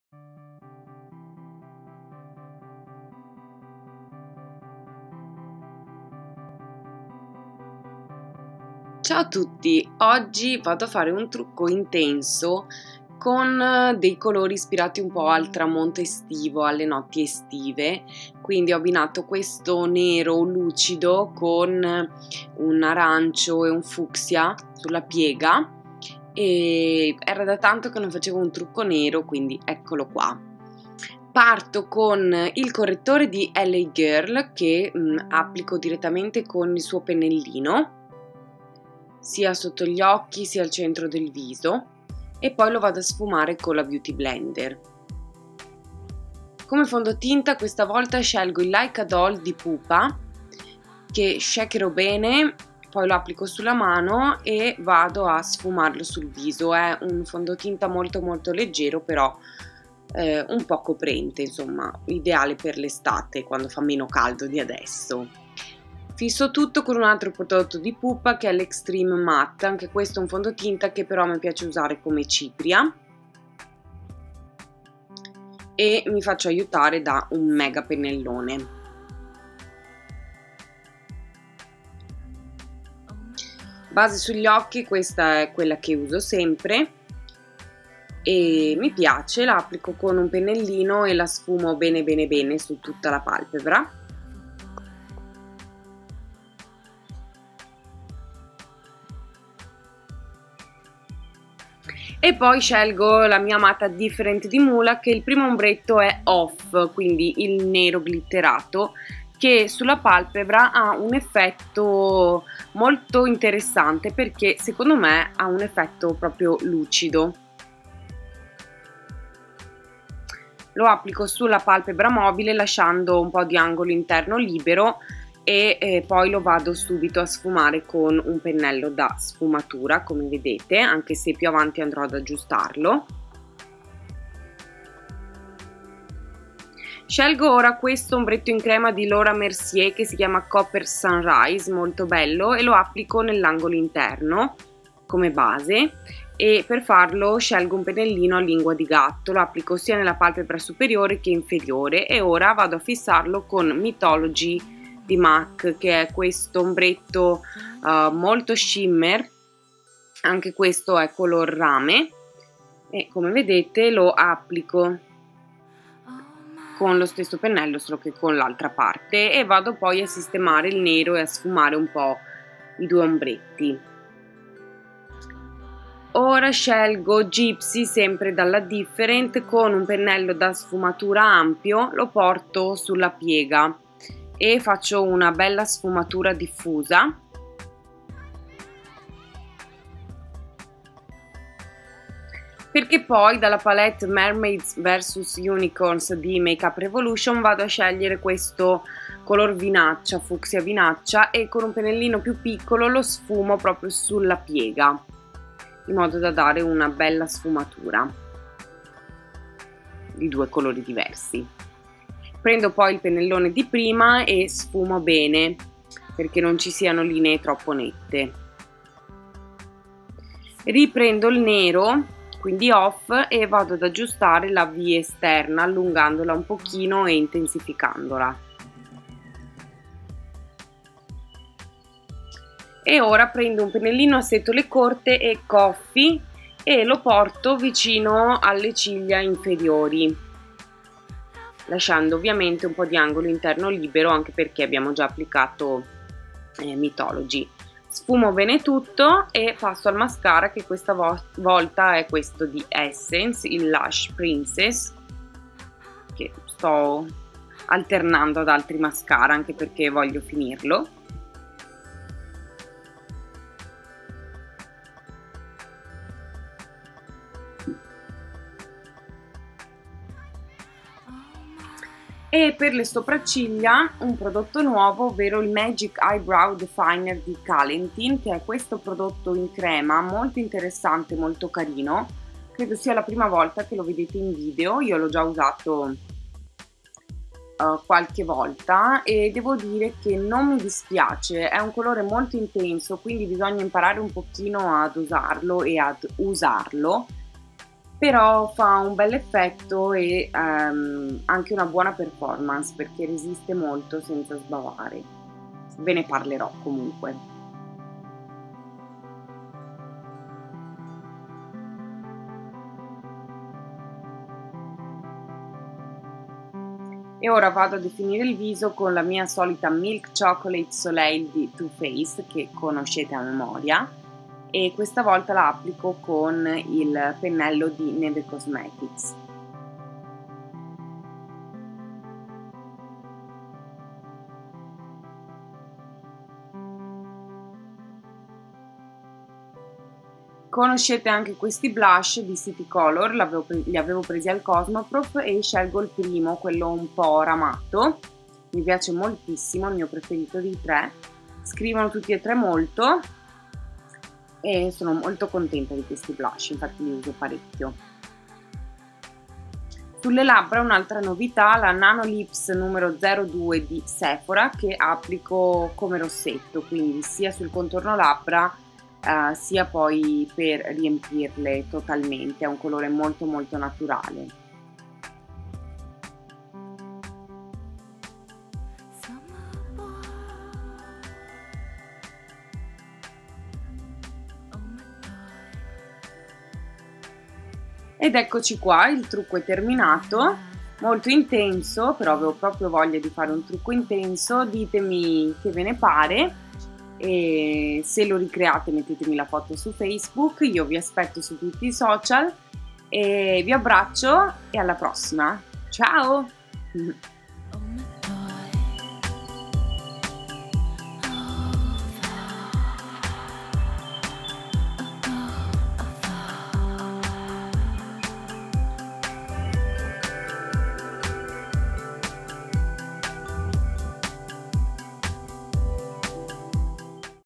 Notti d'estate tut || con prodotti mulac
Ecco un nuovo makeup super intenso per il quale ho usato principalmente prodotti mulac, mi piace perchè mi ricorda le notti d'estate!

♥ aggiungimi su Instagram:
via cellulare cerca GREENFRA1

cerca @greenfra1

musica da Jamendo